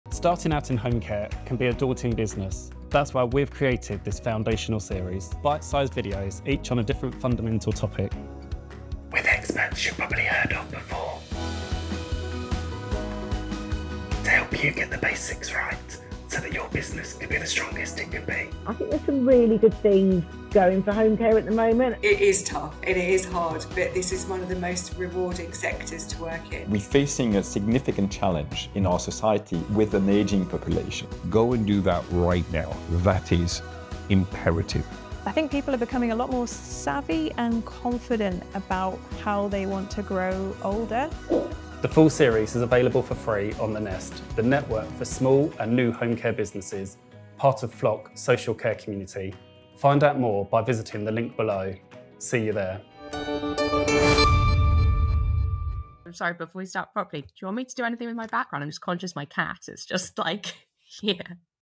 The Foundational Series Trailer
Exclusive to The Nest: we bring you the ultimate foundational series for small domiciliary care businesses in the UK. This 12-part video series has been crafted for the heart of the UK's homecare industry - to help you get the foundations right.

Hosted by award-winning care mentor Mark Topps, each session invites you into an intimate conversation with guest experts, from Amrit CEO at Walfinch, to Camille Leavold, MD at Abbots Care, unravelling the intricacies of domiciliary care businesses, and how to nurture or sustainably grow your smaller care business.

Would you like exclusive access to the entire 12-part series? Sign up to The Nest, the exclusive network for small businesses in homecare today